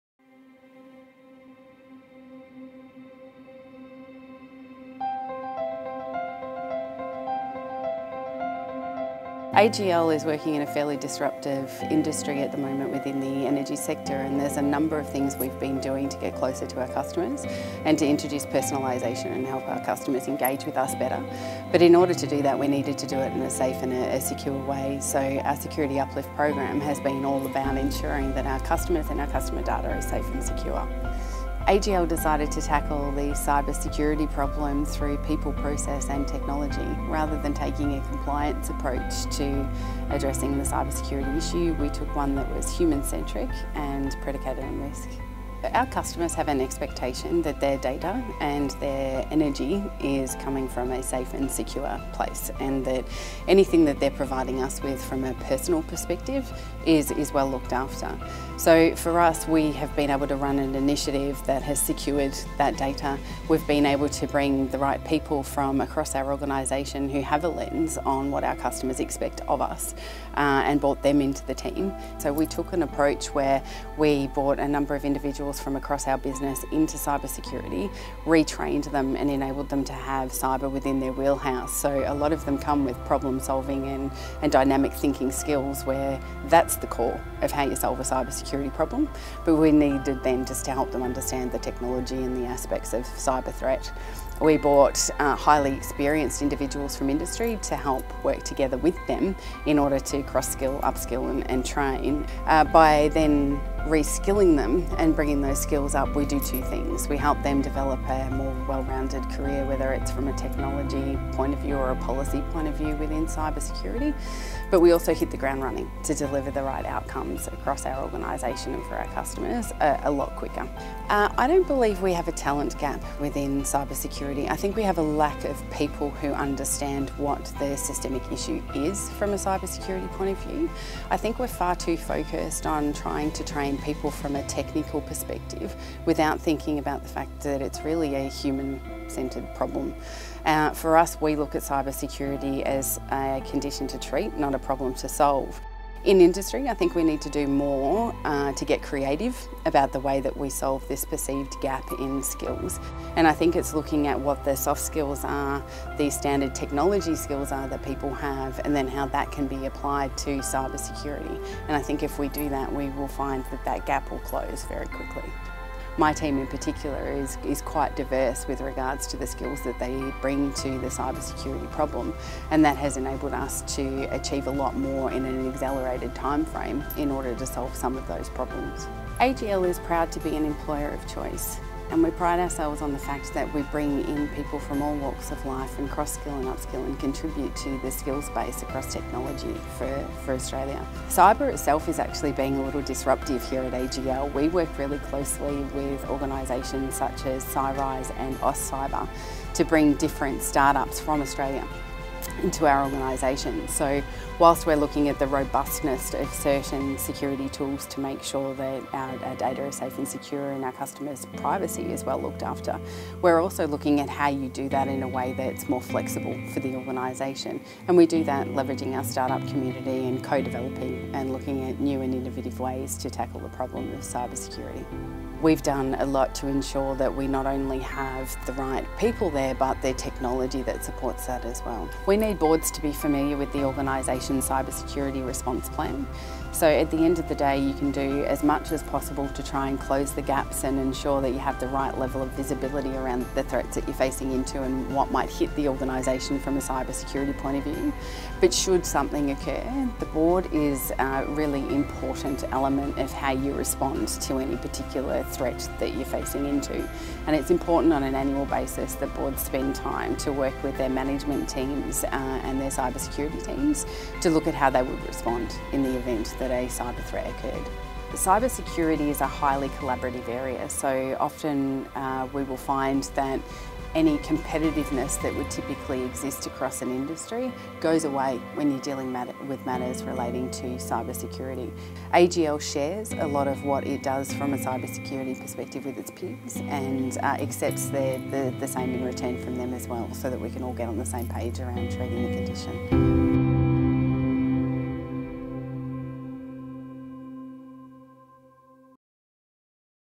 Create, Disrupt, Change: AGL's Security Uplift Program, keeping Australia safe
Samantha MacLeod, CISO, AGL, discusses how industry disruption and evolution of the energy sector have brought new cybersecurity challenges.